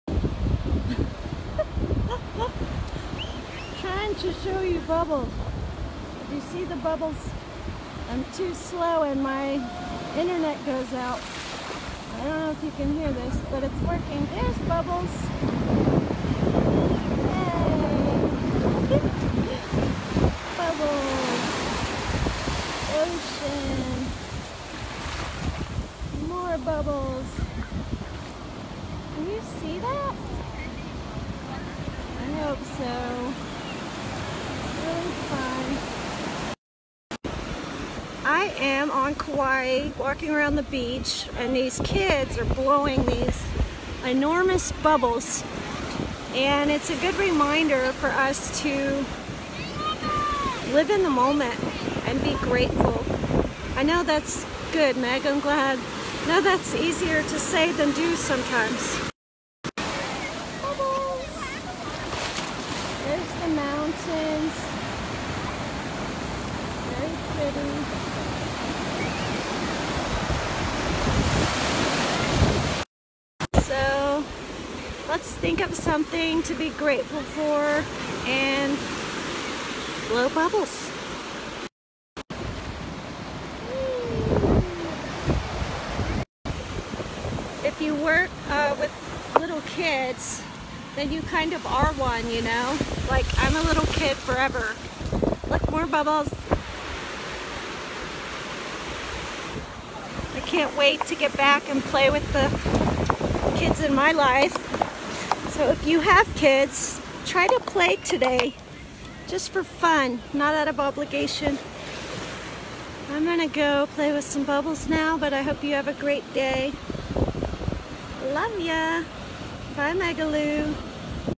Check out bubbles on Kauai! Fun and gratitude!
I was strolling the beach in Kauai Dec. 5, 2018 and stumbled upon some big bubbles. You would think after being a nanny for 20 years I'm no longer into bubbles- but I totally am! I hope this makes you smile!